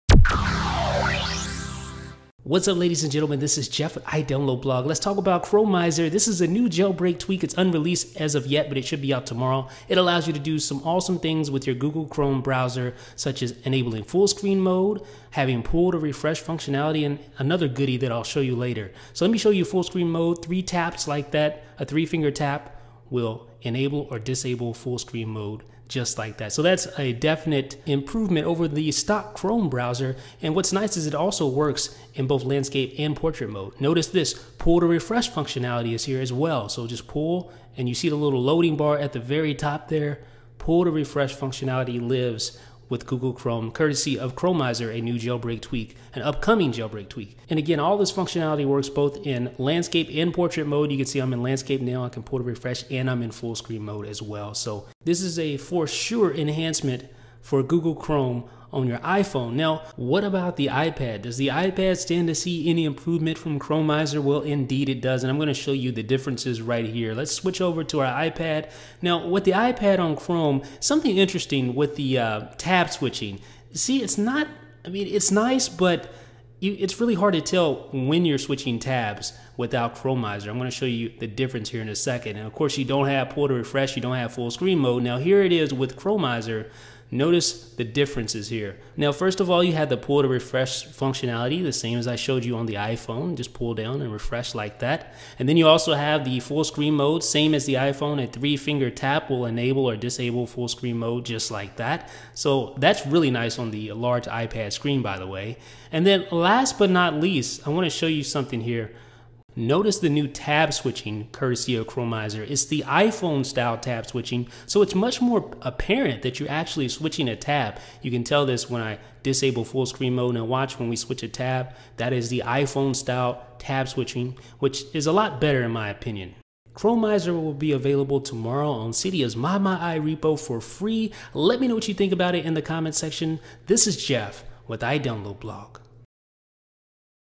Chromizer
Name: Chromizer
Description: Add full screen mode, pull to refresh, and iPhone tab switching to Google Chrome on iOS.
Price: Free
Repo: ModMyi
Developer: cjori

About iDB